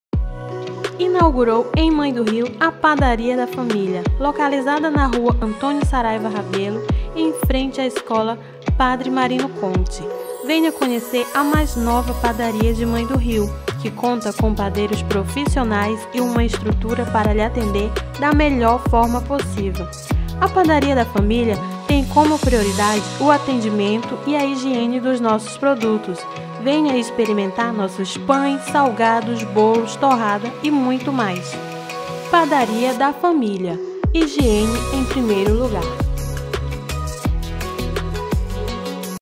PADARIA DA FAMÍLIA MÃE DO RIO - INAUGURAÇÃO
PADARIA DA FAMÍLIA.
Localizada na rua Antônio Saraiva, bem em frente a escola Pé. Marino Contti.
Venha conhecer a padaria da família 🥳😋